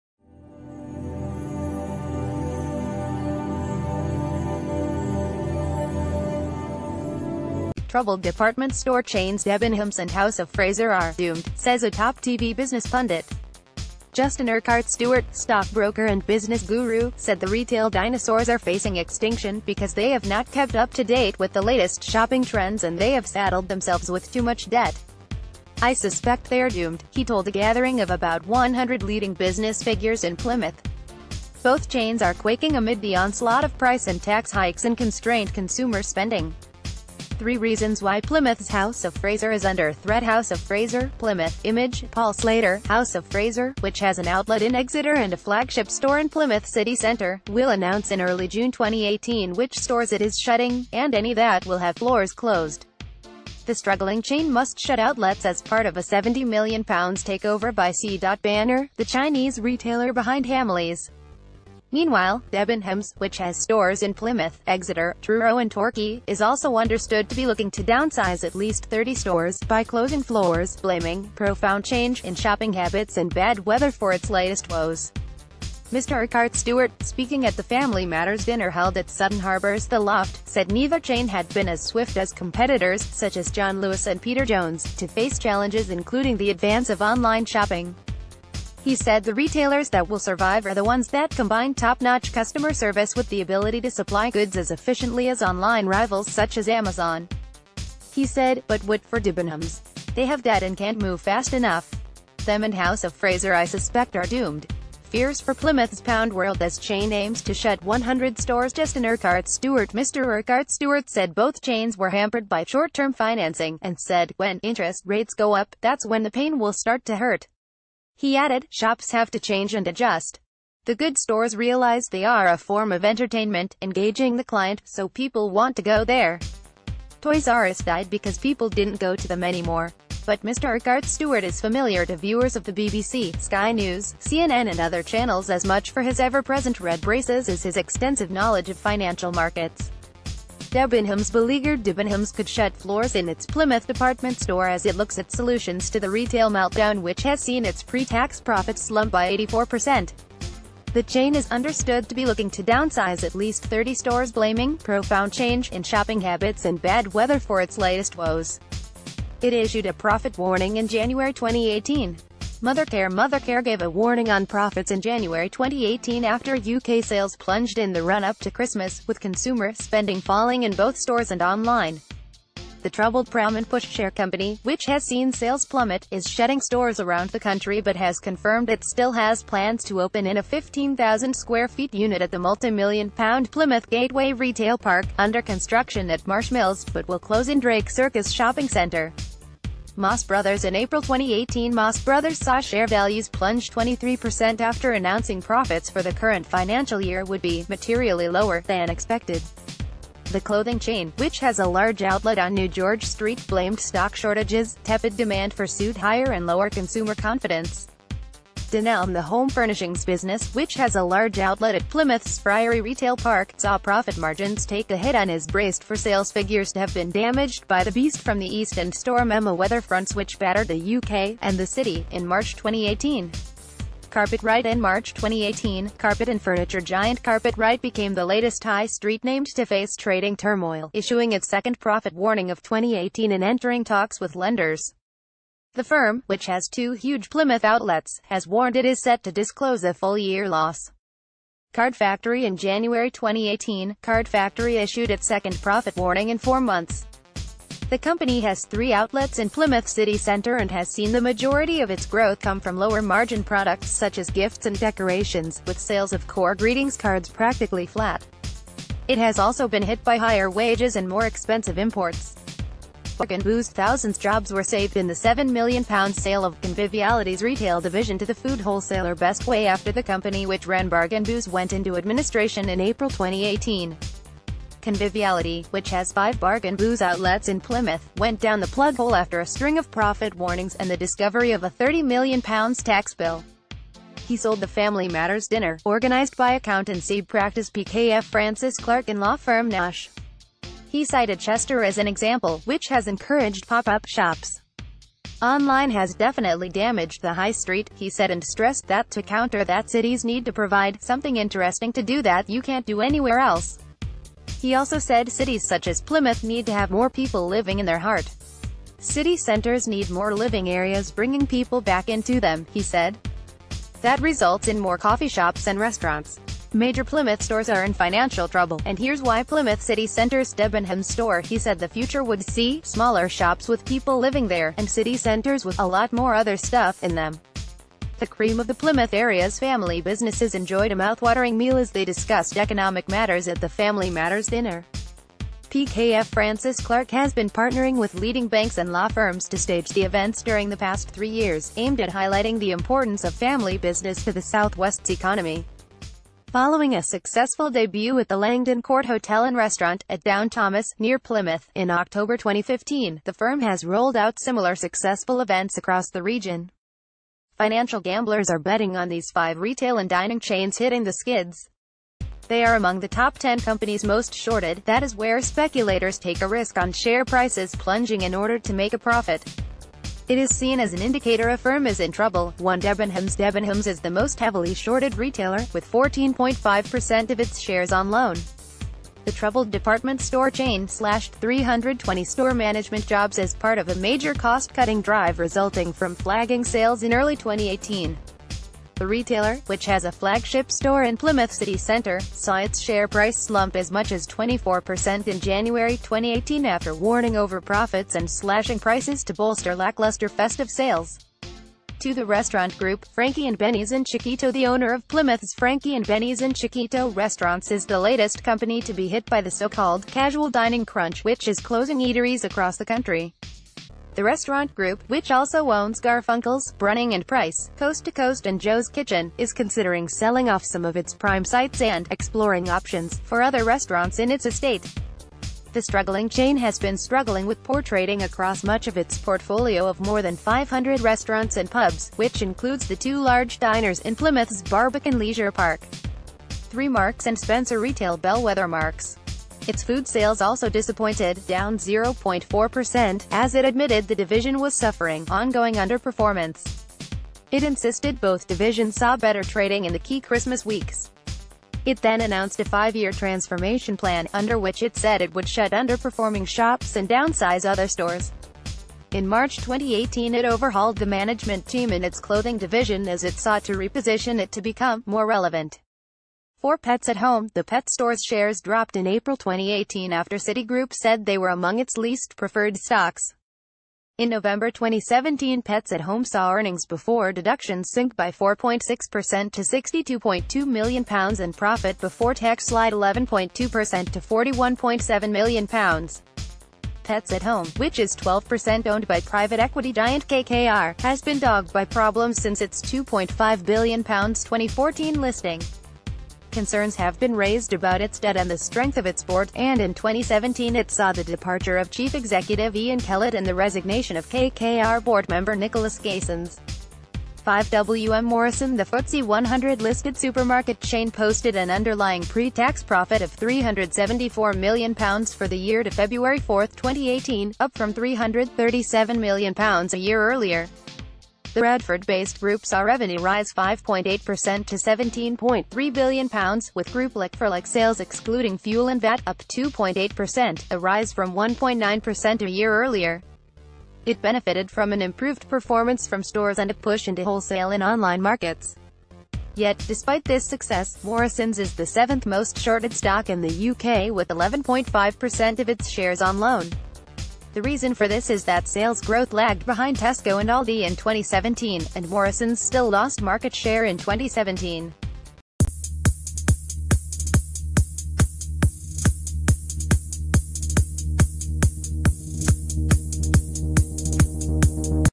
Breaking News | Debenhams and House of Fraser are 'doomed' says TV expert
Troubled department store chains Debenhams and House of Fraser are “doomed”, says a top TV business pundit. Justin Urquhart Stewart, stockbroker and business guru, said the retail dinosaurs are facing extinction because they have not kept up to date with the latest shopping trends and they have saddled themselves with too much debt. “I suspect they are doomed,” he told a gathering of about 100 leading business figures in Plymouth. Both chains are quaking amid the onslaught of price and tax hikes...